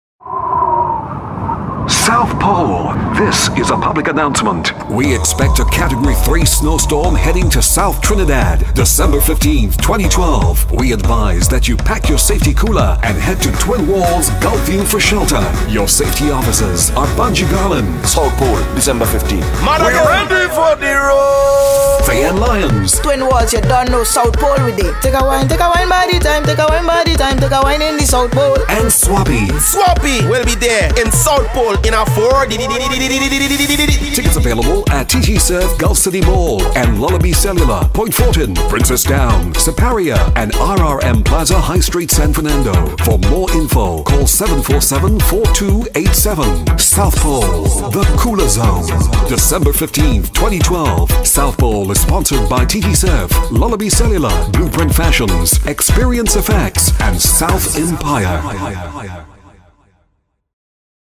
South Pole final video
South Pole is a cooler event on December 15th at Twin Walls featuring Bunji Garlin, Fay-ann Lyons & Swappi